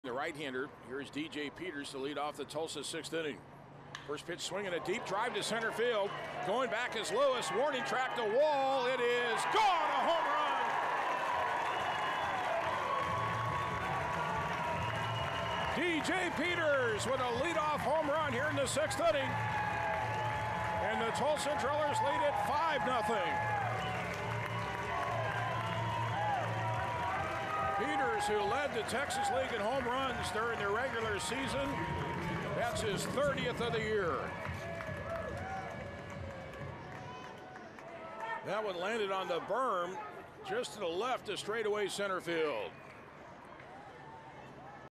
Peters crushes solo shot for Tulsa
9/9/18: DJ Peters crushed a solo shot in the sixth inning for the Drillers to extend lead to 5-0